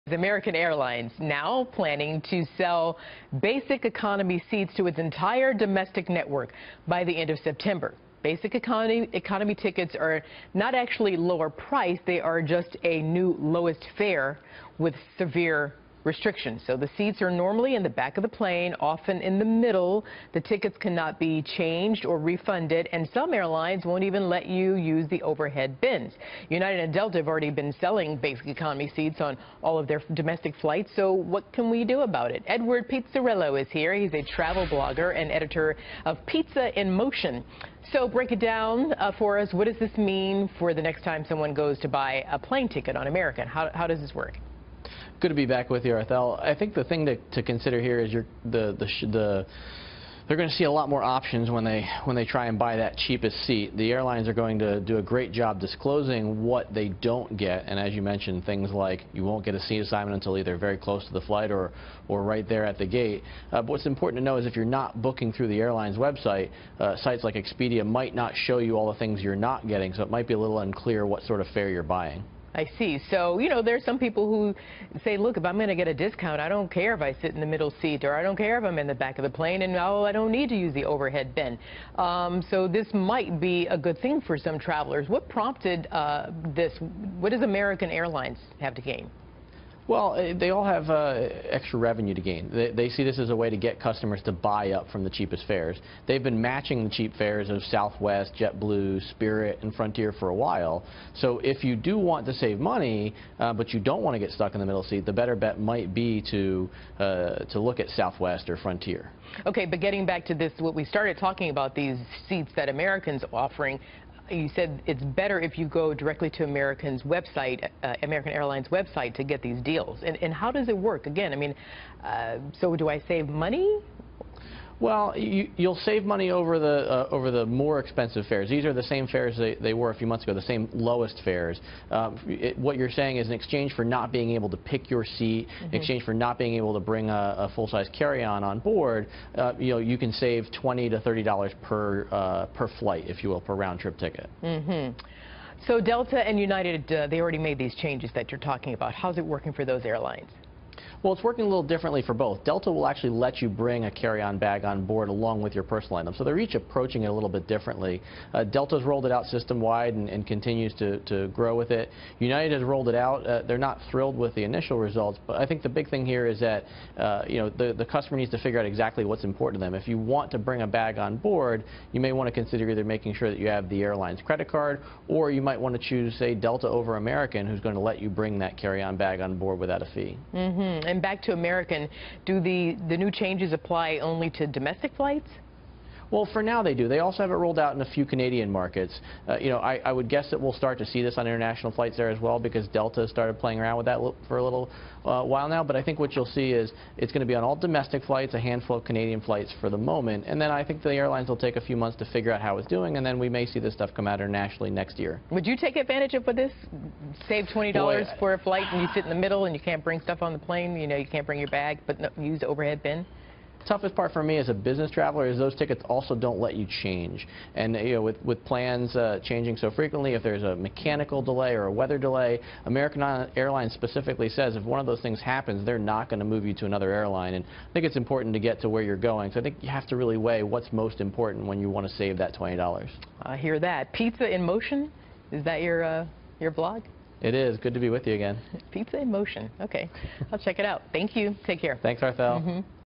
American Airlines to take 'basic economy' option nation wide
Edward Pizzarello describes the details of the new program